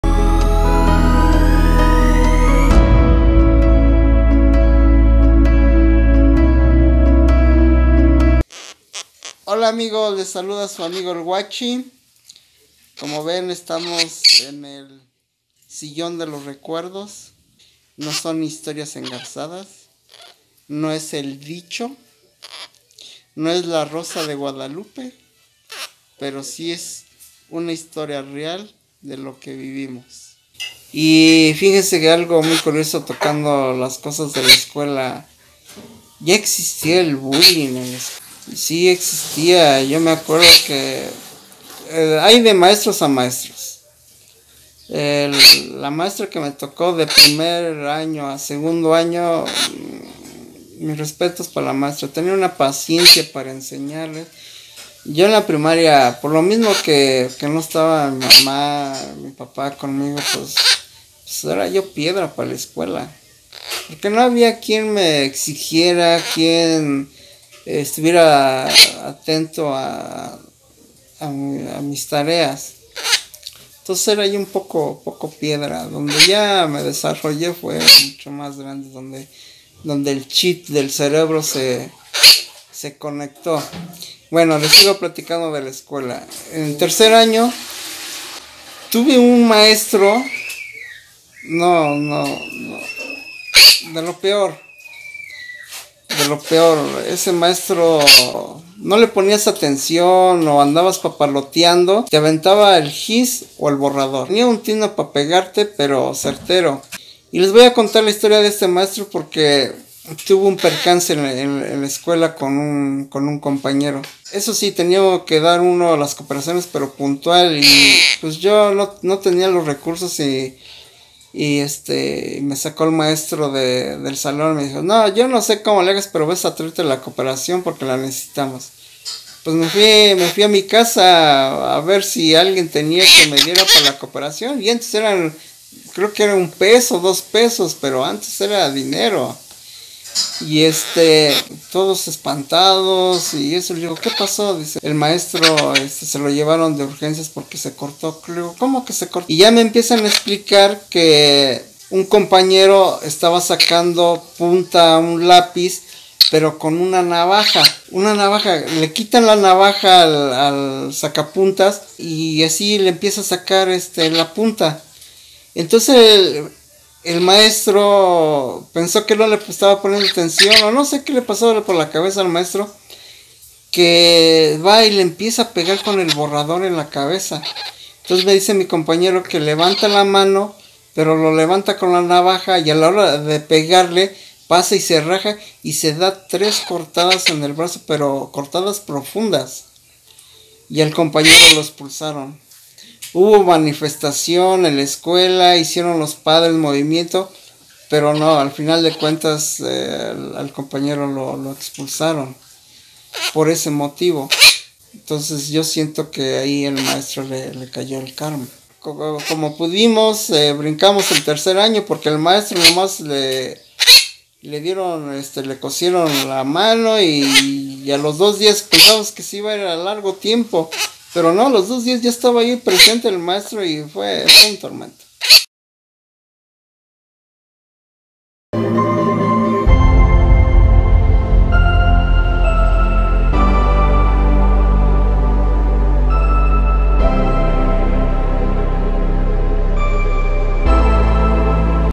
El sillón de los recuerdos - Capitulo 02
Hola Huachis en este nuevo capitulo les platico como viví la escuela, espero que les agrade.